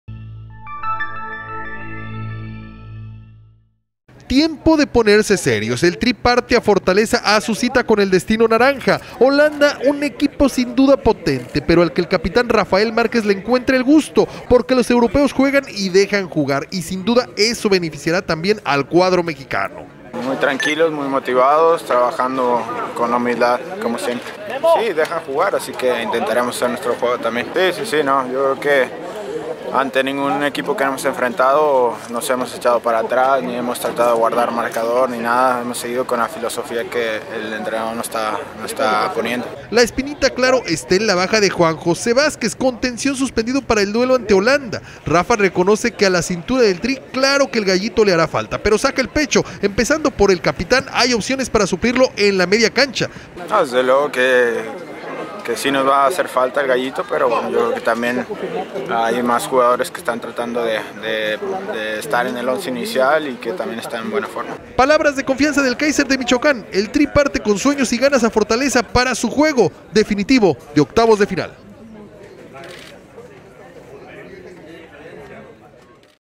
"Resentiremos la baja del Gallito": Márquez
BRASIL 2014. Santos, Brasil. 27/06/2014. Tiempo de ponerse serios, el Tri parte a Fortaleza a su cita con el destino naranja: Holanda un equipo, sin duda potente, pero al que el capitán Rafael Márquez le encuentra el gusto por enfrentar.